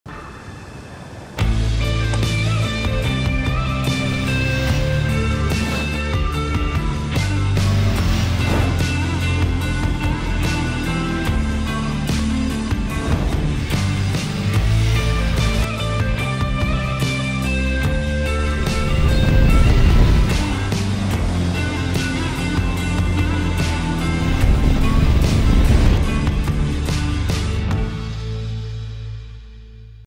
O'NEAL M-SRS Touring Motorcycle Helmet
The O'NEAL M-SRS HELMET - a brand new helmet for your next touring adventure. The main highlight of this helmet is the functional and easy-to-use flip-up mechanism. Perfect for all riders who appreciate the convenience this offers on and off the bike.

The features of the O'NEAL M-SRS Touring Motorcycle Helmet:

• Helmet with 2 outshells and 2 EPS for increased safety and perfect fit
• Shell is produced with ABS
• Multiple air vents for optimal ventilation and cooling
• Padded comfort liner, removable, sweat absorbing and washable
• Microlock closure
• Bluetooth Ready with internal speaker cavities and rubber hoop at back of the helmet (Bluetooth unit not included)
• Prepared for Pinlock® 70 MaxVision fog resistant insert lens
• Clear Visor plus secondary integrated sun visor
• Solid Flip-Up mechansim with easy handling and smooth function
• Meets ECE 22.05 safety standards